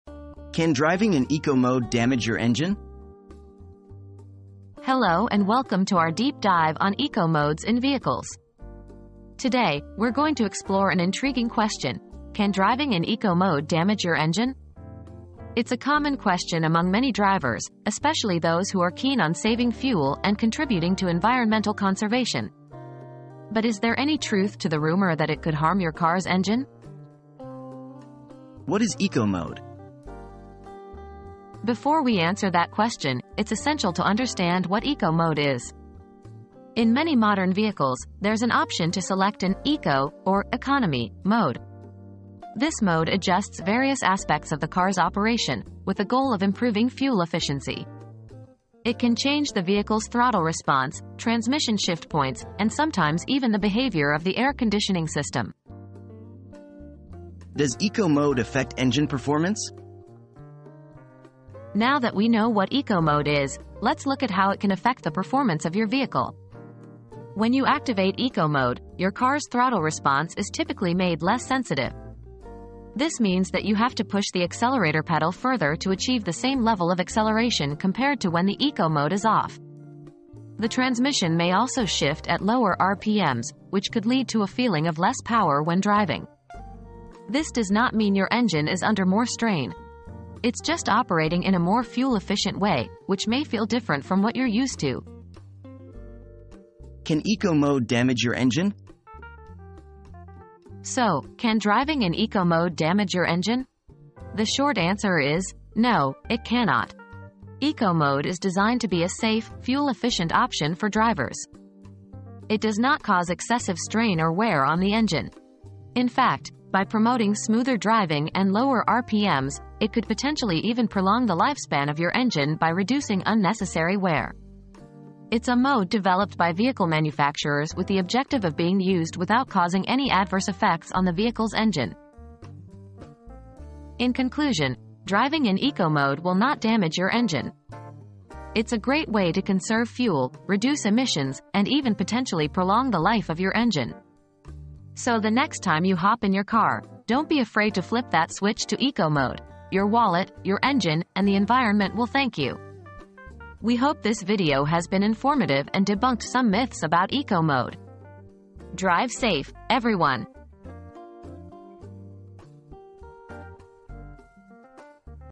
Can driving in Eco mode damage your engine?
Does Eco Mode Harm Your Engine? • Curious about whether driving in Eco mode can harm your engine? In this captivating video, we explore this intriguing question and debunk any myths surrounding it. Discover what Eco mode is and how it affects engine performance, while also learning why it is actually designed to be a safe and fuel-efficient option. Join us for this deep dive into the world of Eco mode and find out how it can potentially prolong the lifespan of your engine!

00:00 • Can driving in Eco mode damage your engine?
00:28 • What is Eco Mode?
00:57 • Does Eco Mode Affect Engine Performance?
01:37 • Can Eco Mode Damage Your Engine?